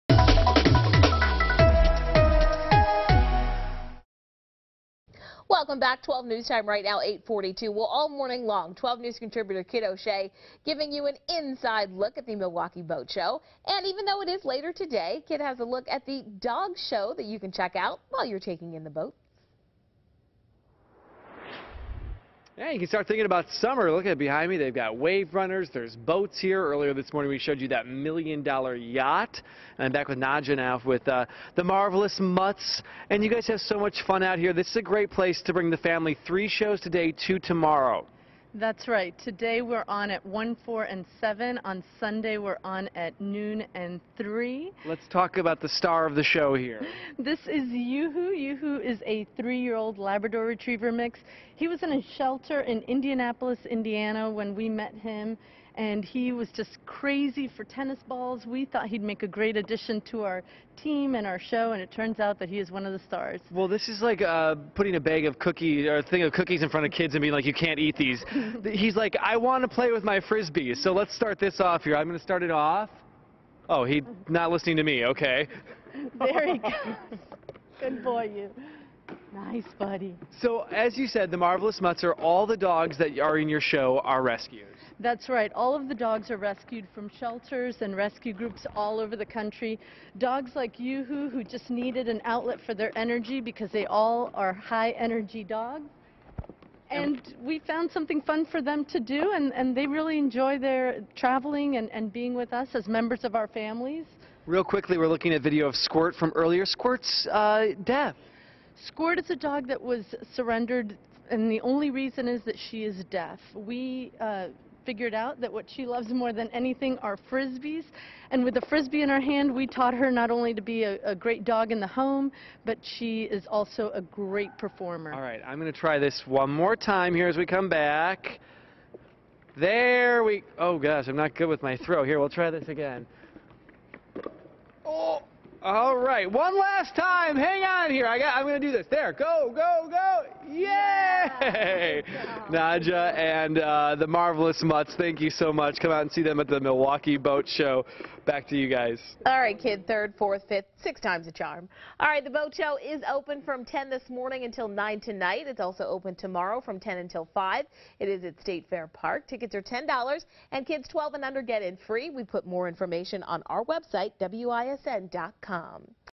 Dog Show at the Boat Show
Contributor Kidd O'Shea stops by the Milwaukee Boat Show and checks out the Dog Show.